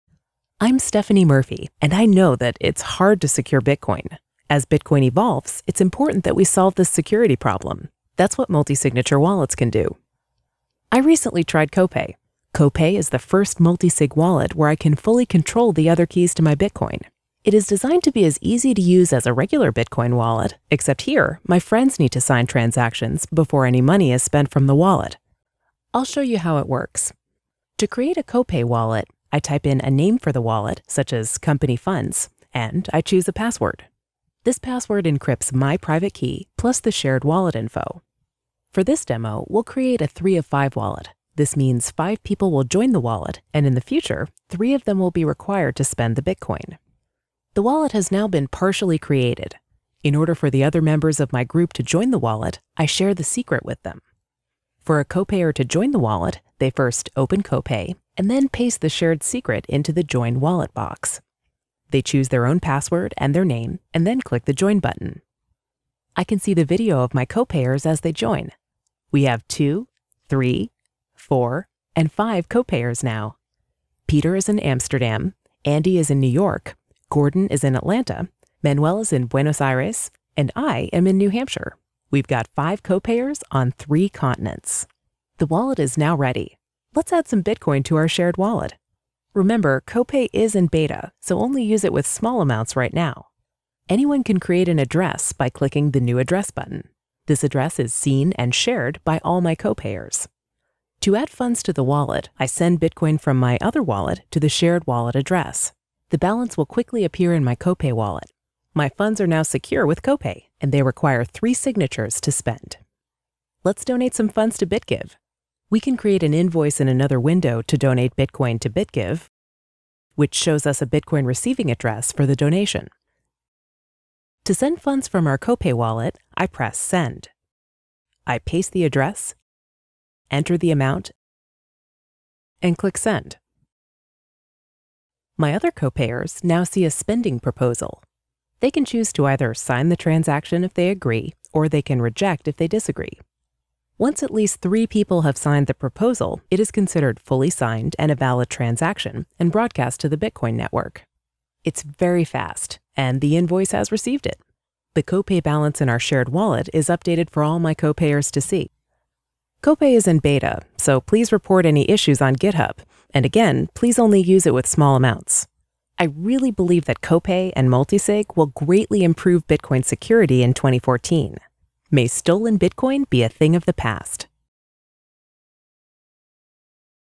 BitPay Announces Copay, a Multi-Signature Bitcoin Wallet
BitPay, the leading payment processor for Bitcoin transactions is happy to announce a multi-signature wallet, Copay. Unlike other "Multi-User Accounts", Copay is truly a Multi-Signature Wallet that gives you and your Copayers compete control of the private keys needed to access your bitcoin.